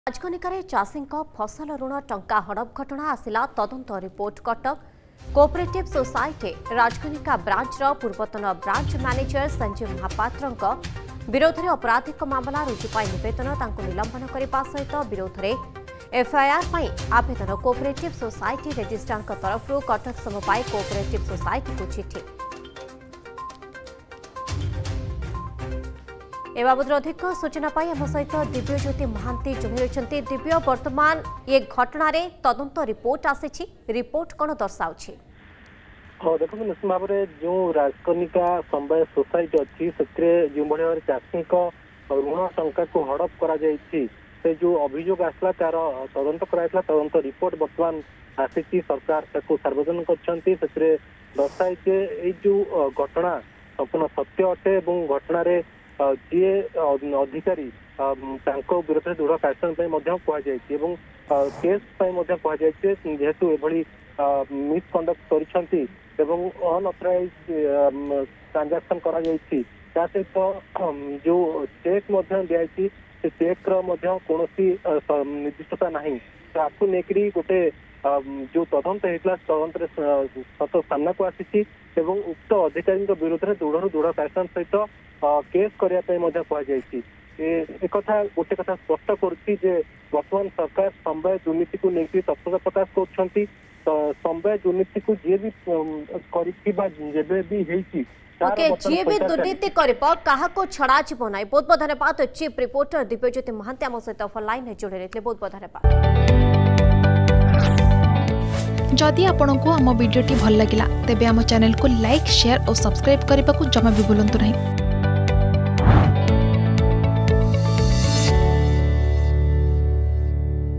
ରାଜକନିକାରେ ଚାଷୀଙ୍କ ଫସଲ ରୁଣ ଟଙ୍କା ହଡପ ଘଟଣା | Rajkanika Farmers Issue
ରାଜକନିକାରେ ଚାଷୀଙ୍କ ଫସଲ ରୁଣ ଟଙ୍କା ହଡପ ଘଟଣା । ରୁଣ ଟଙ୍କା ହଡପ ଘଟଣାରେ ଆସିଲା ତଦନ୍ତ ରିପୋର୍ଟ । ବ୍ରାଞ୍ଚ ମ୍ୟାନେଜର ସଞ୍ଜିବ ମହାପାତ୍ରଙ୍କ ବିରୋଧରେ ଅପରାଧିକ ମାମଲା ରୁଜୁ ପାଇଁ ନିବେଦନ । ନିଲମ୍ବନ କରିବା ସହିତ ମ୍ୟାନେଜରଙ୍କ ବିରୋଧରେ ଏଫଆଇଆର ପାଇଁ ନିବେଦନ ।